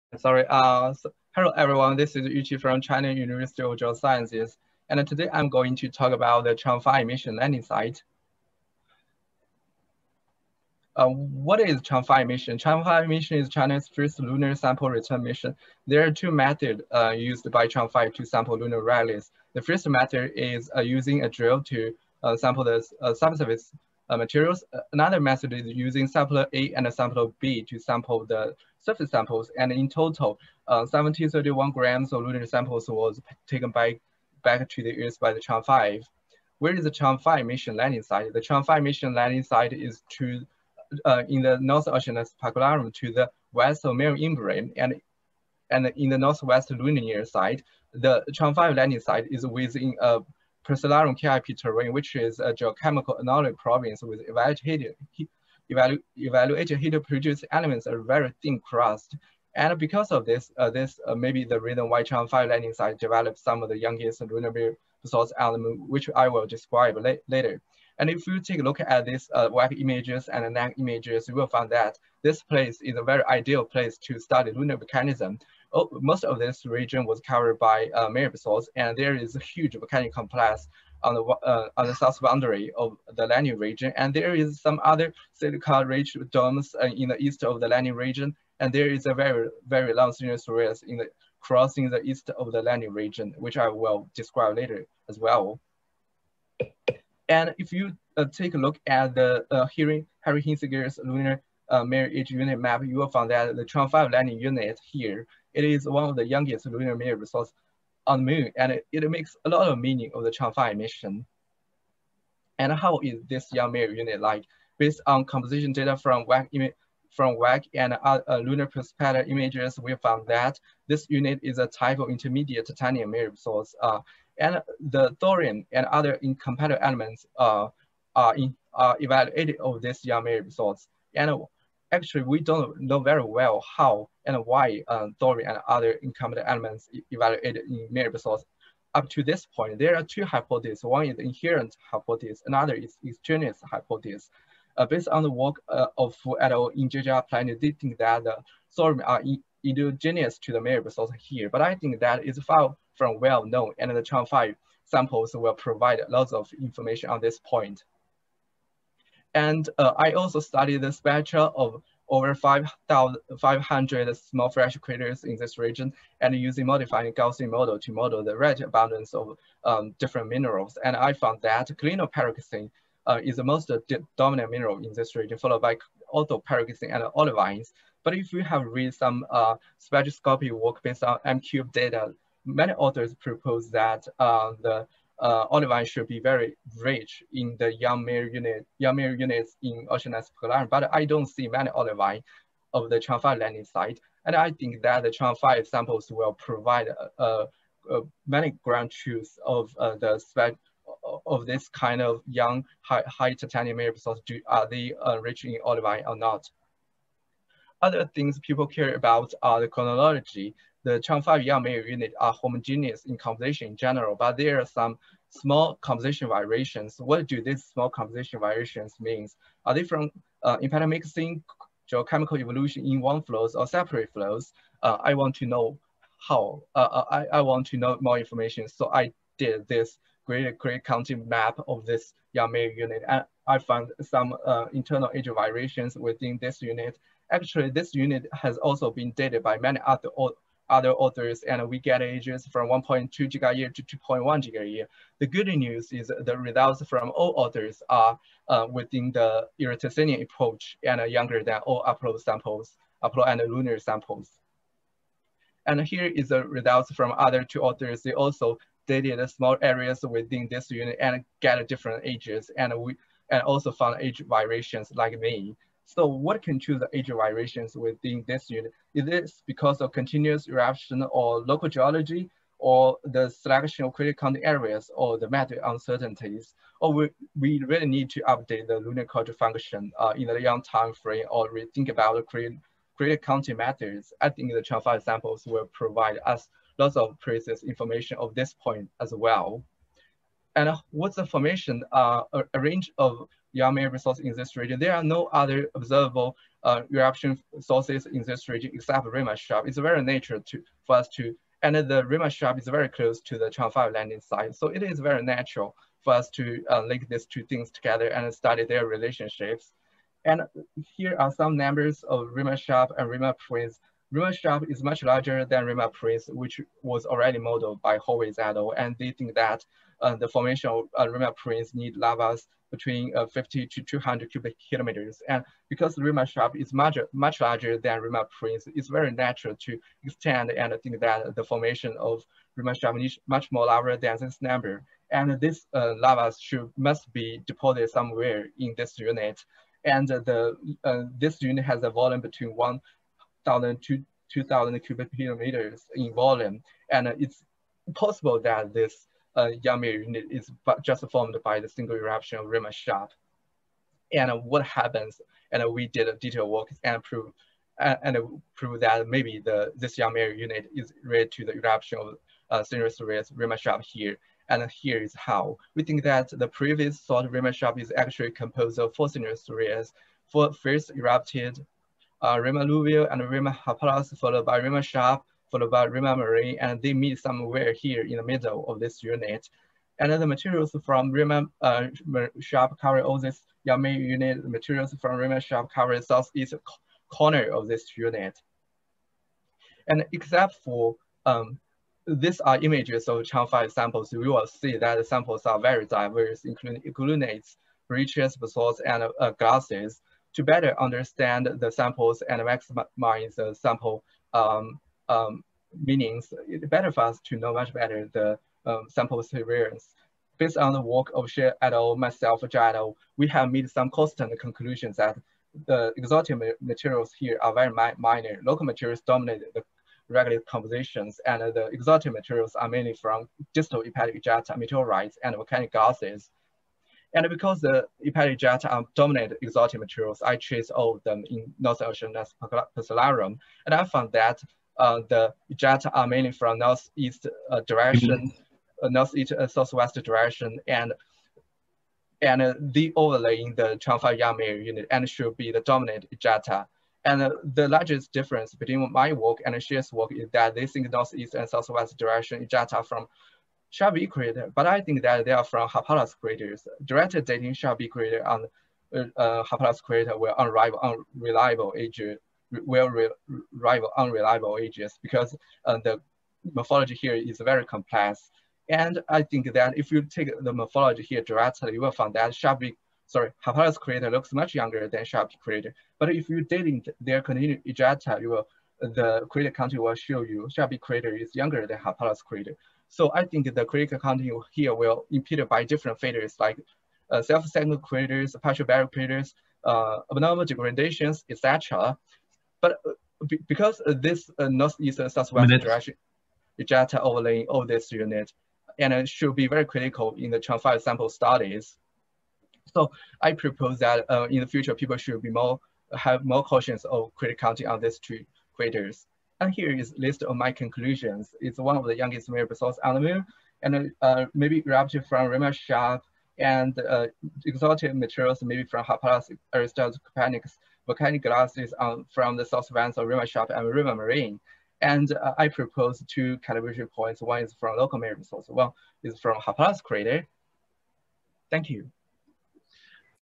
Chang'e 5 Landing Site: An Overview (July 22, 2021)
This is my 2021 NASA Exploration Science Forum & European Lunar Symposium presentation on July 22nd, 2021, entitled "Chang'e 5 Landing Site: An Overview". The corresponding abstract is available through this